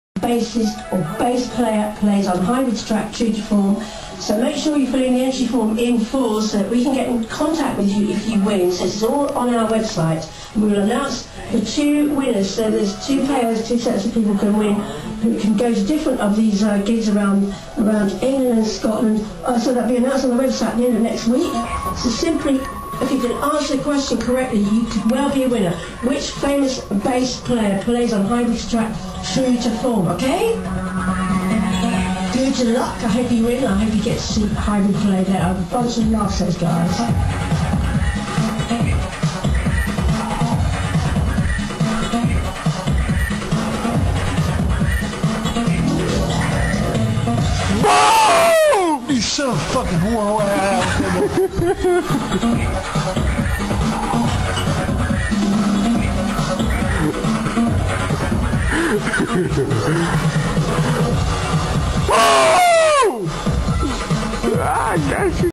Boo Sam
Sneak attack on Sam while he was passed out by James

BillyP